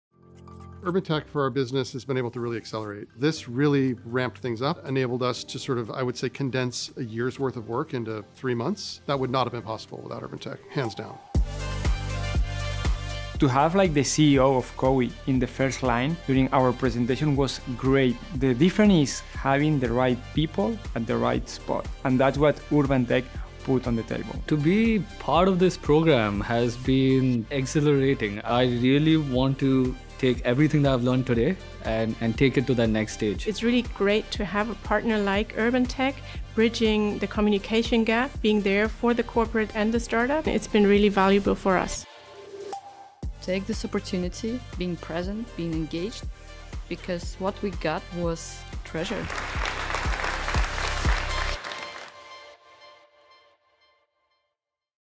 What can Urbantech do for a startup | Demo Day 2019
We are very excited to see all the great results from the Urbantech program, that we have hosted for the last three months.

Urbantech is a global innovation program, connecting growth-stage startups with leading corporates within Urbantech. The carefully chosen tech startups have worked together from the BLOXHUB co-working space and with the BLOXHUB Community. Now the program has come to an end and today they will present their work for partners and press. It’s been a learning process for all involved and definitely a success to be repeated next year.